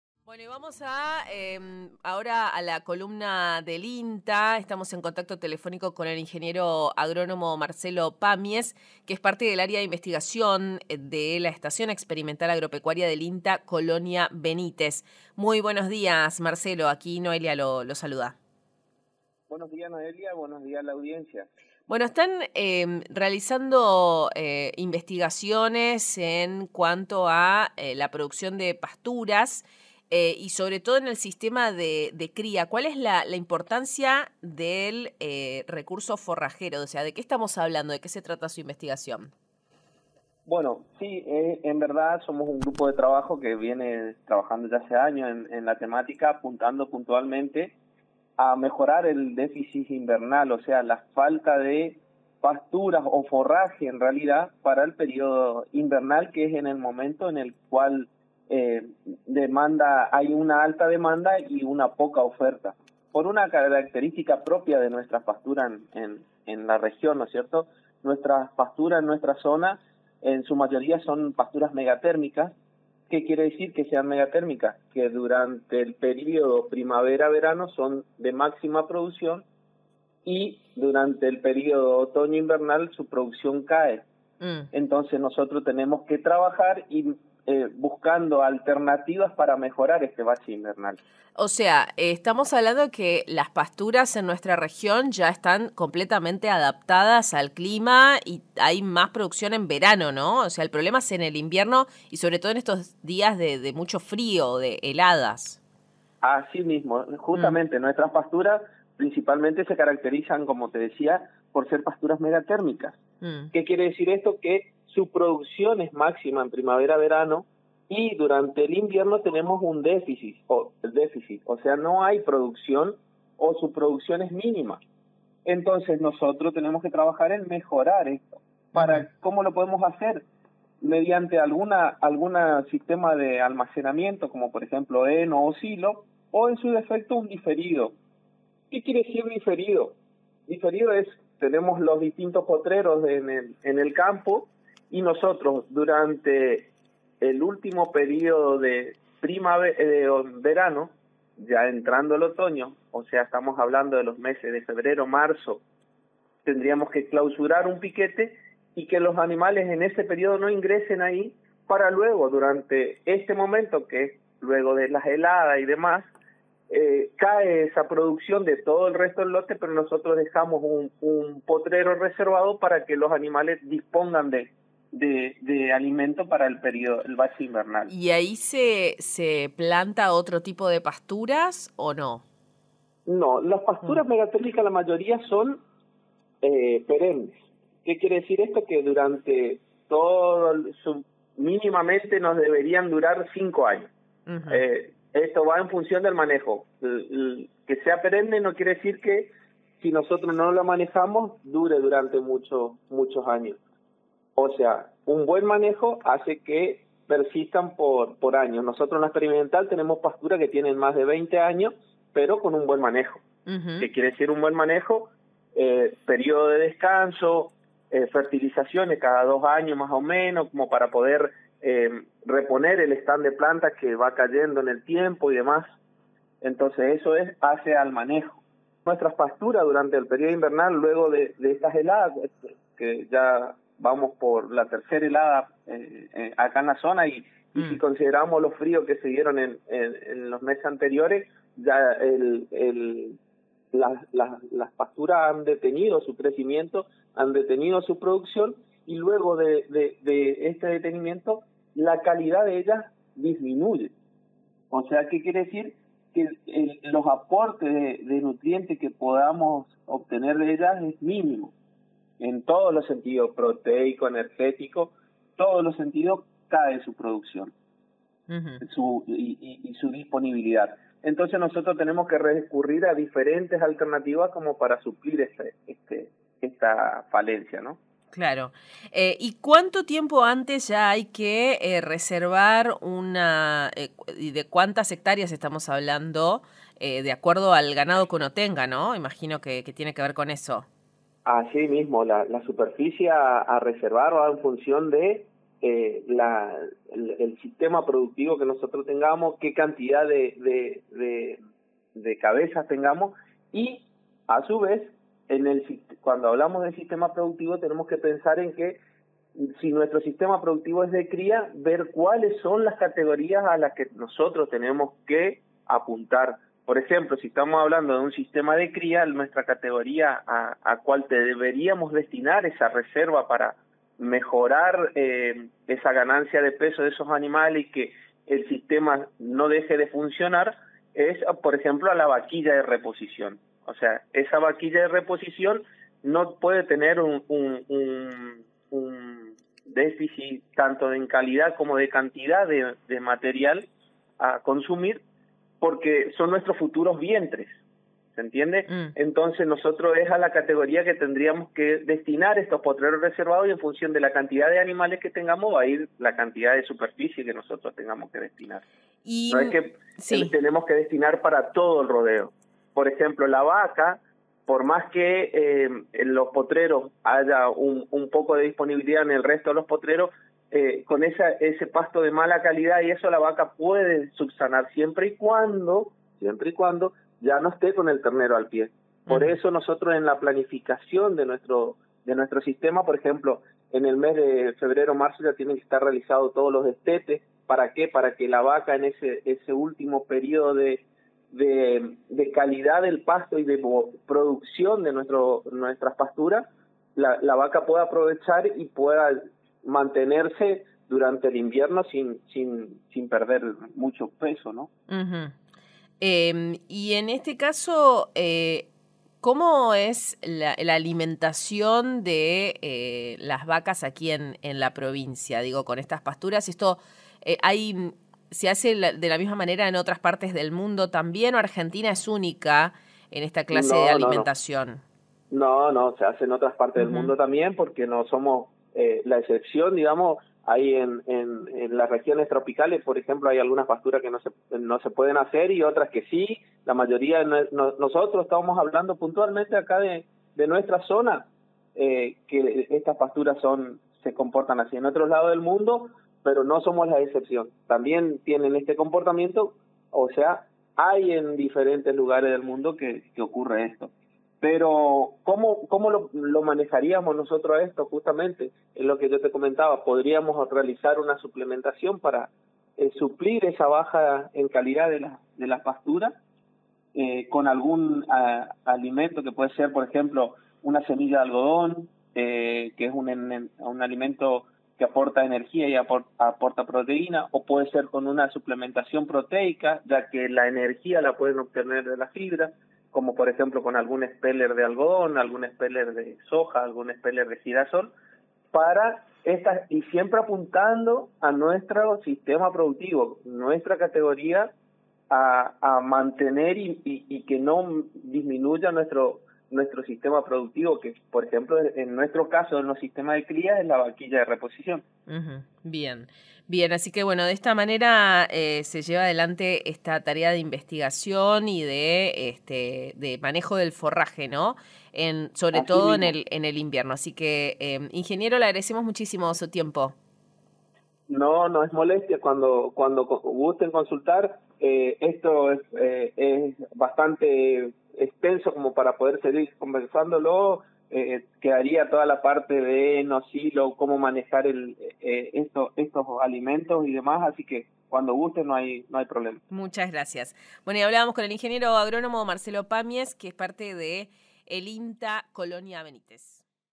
𝐈𝐍𝐓𝐀 | 𝐌𝐚𝐧𝐞𝐣𝐨 𝐈𝐧𝐯𝐞𝐫𝐧𝐚𝐥 𝐝𝐞𝐥 𝐫𝐞𝐜𝐮𝐫𝐬𝐨 𝐟𝐨𝐫𝐫𝐚𝐣𝐞𝐫𝐨, 𝐞𝐧 𝐞𝐥 𝐬𝐢𝐬𝐭𝐞𝐦𝐚 𝐝𝐞 𝐜𝐫𝐢́𝐚
📌Ganadería 🎙 El Ing. Agr. Marcelo Pamies – INTA EEA Colonia Benítez, ofreció una entrevista radial al programa “Radio Nacional” 📻 (Radio Nacional Resistencia AM 620 y FM 96.7), sobre la importancia del manejo del recurso forrajero.